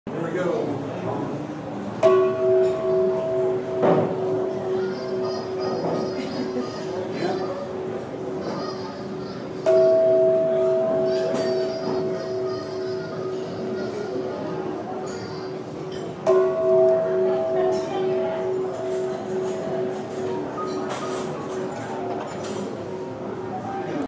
Large chime at the Omni Grove Park Inn's Sunset Terrace restaurant
My brother-in-law striking the large Grove Park Inn Sunset Chime at the Omni Grove Park Inn, in Asheville, NC.